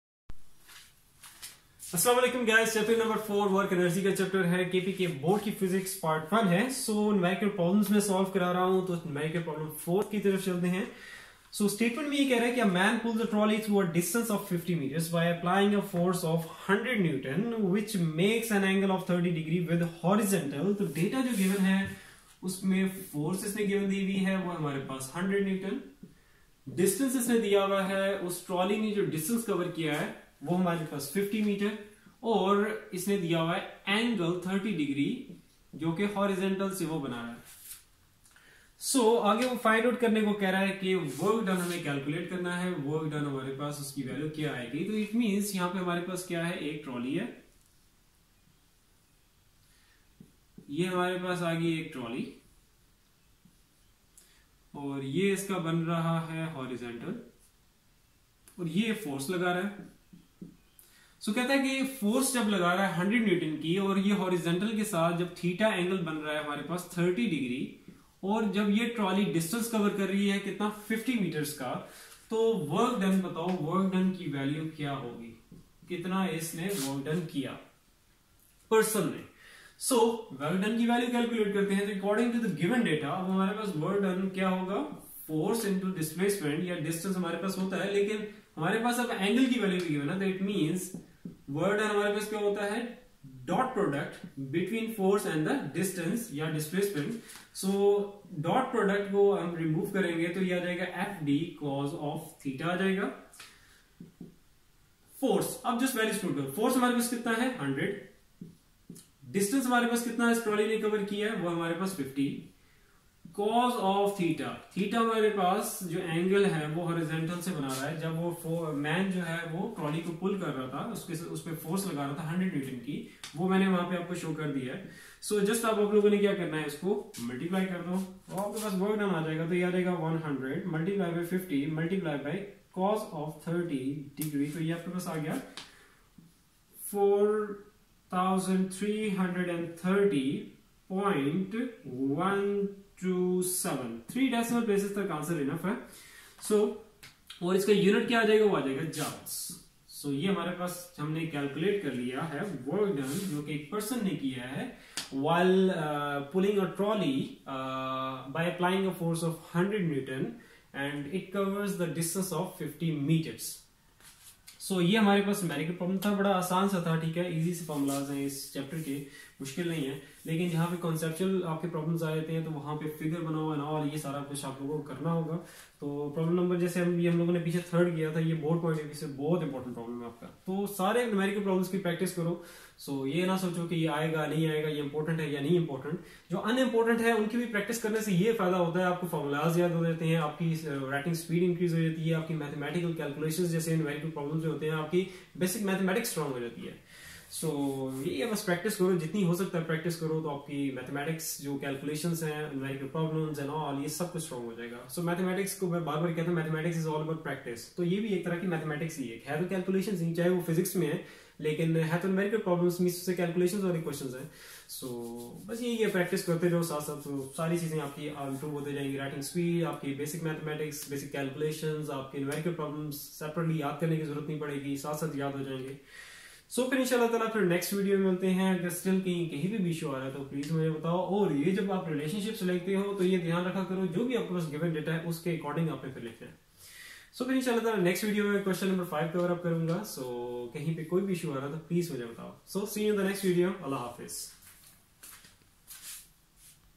kpk fsc physics part1 ch4 p3 | fbise fsc physics part1 ch4 p3
Numerical Problem # 1:
Statement: A 70 kg man runs up a long flight of stairs in 4 s. The vertical height of the stair is 4.5m. Calculate his power.

Numerical Problem # 2:
Statement: A body of mass 2.0 kg is dropped from a rest position 5m above the ground. What is its velocity at height of 3.0 m above the ground?

Numerical Problem # 3:
Statement: A man pulls a trolley through a distance of 50m by applying a force of 100N which makes an angle of 30° with horizontal. Calculate the work done by the man.

Numerical Problem # 4:
Statement: The roller-coaster car starts its journey from a vertical height of 40m on the first hill and reaches a vertical height of only 25m on the second hill, where it slows to a momentary stop. It traveled a total distance of 400m. Determine the thermal energy produced and estimate the average friction force on the car whose mass is 1000kg.

Numerical Problem # 5:
Statement: A man whose mass is 70kg walks up to the third floor of a building which is 12m above the ground in 20s. Find his power in watts and hp.

Numerical Problem # 6:
Statement: To what height can a 400W engine lift a 100kg mass in 3sec?

Numerical Problem # 7:
Statement: A ball of mass 100 g is thrown vertically upward at a speed of 25m/s. If no energy is lost, determine the height it would reach. If the ball only rises to 25m, calculate the work done against air resistance. Also calculate the force of friction.

Numerical Problem # 8:
Statement: An object of mass 1000 g falls from a height of 30m on the sand below. If it penetrates 4cm into the sand, what opposing force is exerted on it by the sand? Neglect air friction.

Numerical Problem # 9:
Statement: A body of mass ‘m’ drops from bridge into water of the river. The bridge is 10m high from the water surface.
(a). Find the speed of the body 5m above the water surface.
(b). Find the speed of the body before it strikes the water.

Numerical Problem # 10:
Statement: The engine of JF-Thunder fighter (made by Pakistan and China) develops a thrust of 3000N. What horsepower does it at a velocity of 600 m/s.

Numerical Problem # 11:
Statement: The mass of the moon is 1/80 of the mass of the earth and corresponding radius is 1/4 of the earth. Calculate the escape velocity on the surface of moon.